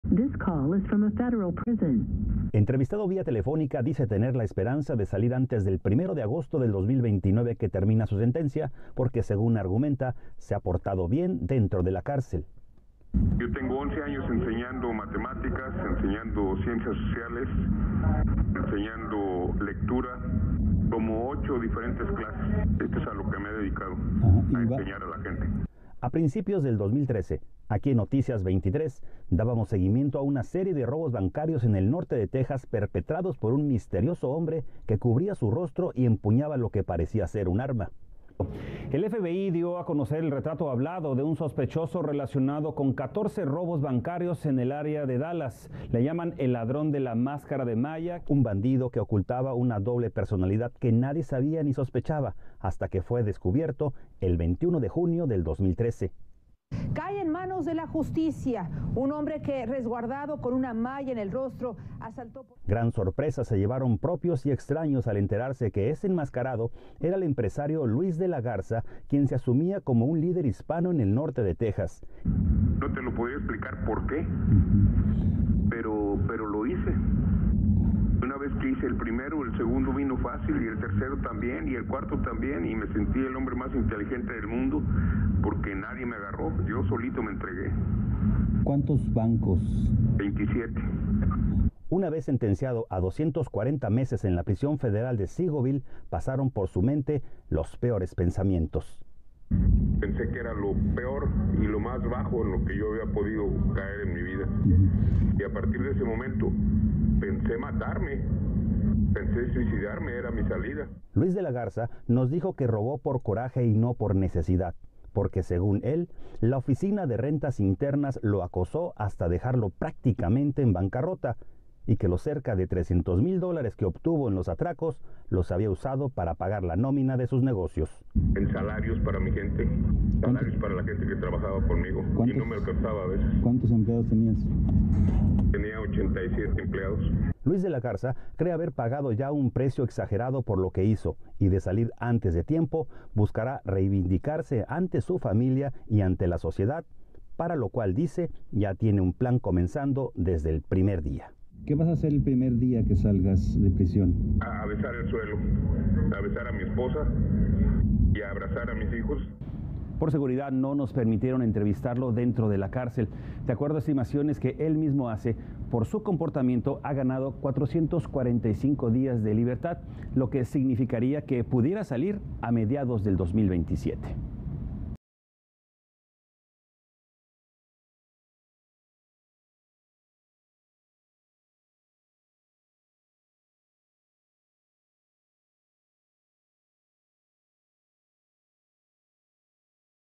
Exlíder hispano Luis de la Garza habla desde la cárcel tras 11 años preso por robar bancos en Texas
Luis de la Garza, exempresario y exlíder hispano en el Metroplex, lleva 11 años en prisión y espera salir antes del 1 de agosto del 2029, fecha en la que termina su sentencia, pues según él ha tenido un buen comportamiento en la cárcel. De la Garza admite haber robado 27 bancos y asegura que él podría salir de la cárcel a mediados del 2027.